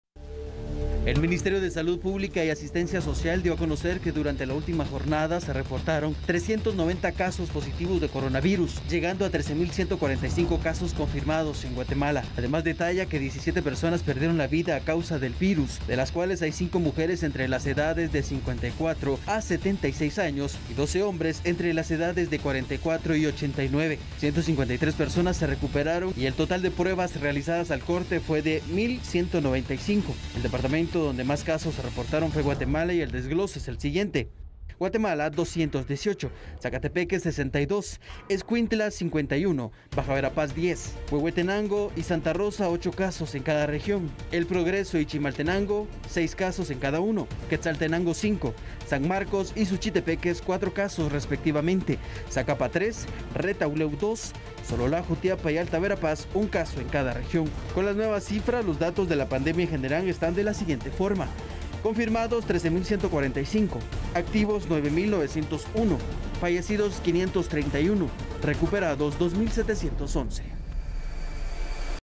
Ministerio de Salud reporta 390 casos nuevos de Covid-19
Al momento se reportan un total de 13 mil 145 casos confirmados en el país
Ya se contabilizan 531 fallecidos en todo el territorio nacional por causa del virus. Al momento se mantienen activos un total de 9 mil 901 casos.